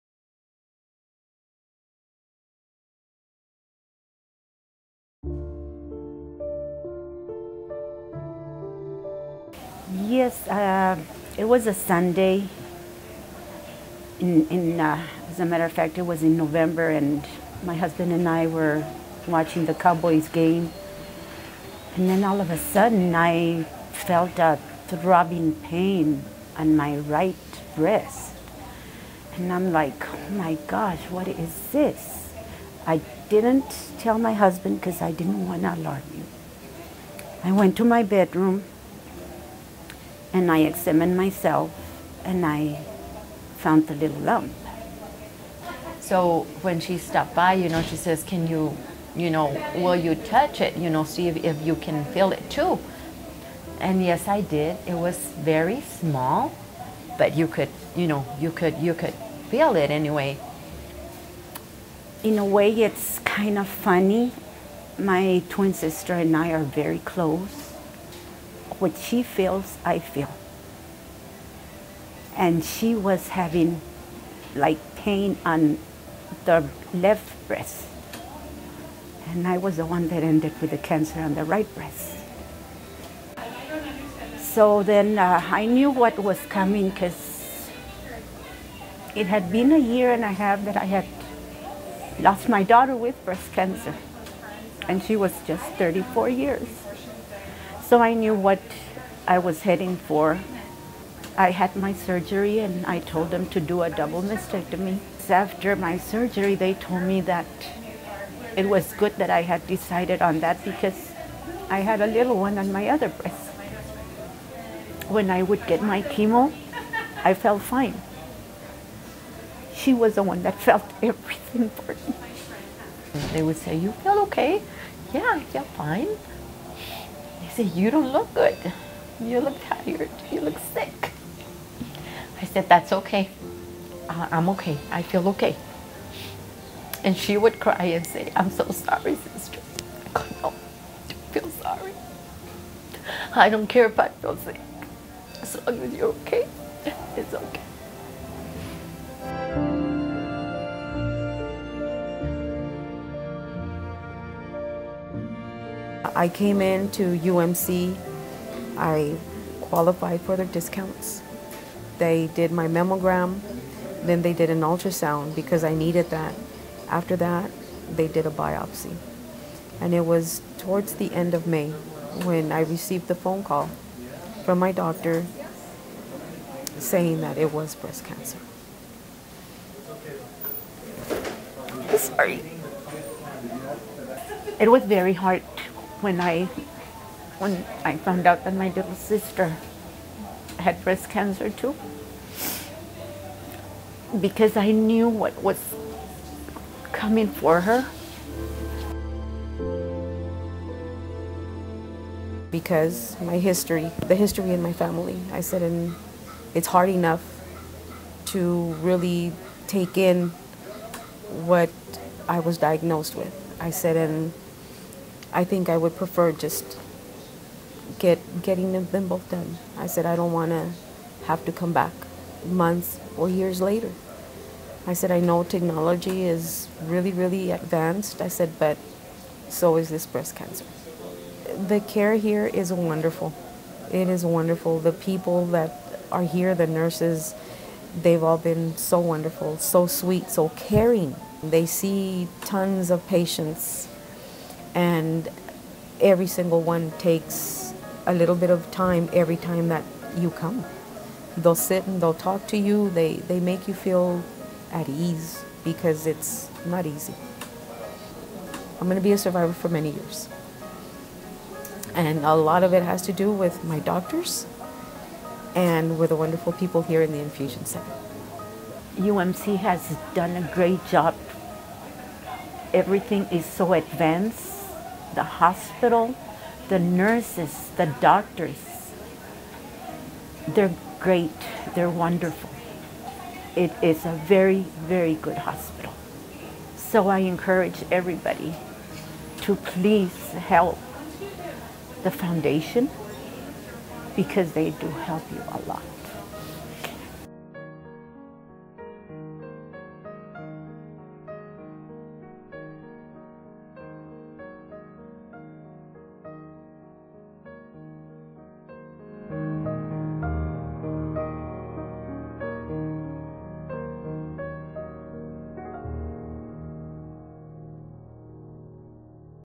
Rita Bristow's Story
Rita Bristow comes from a family with history of breast cancer. When she was diagnosed with breast cancer, she decided to request a double mastectomy. This is the story on how University Medical Center of El Paso helped her and her family.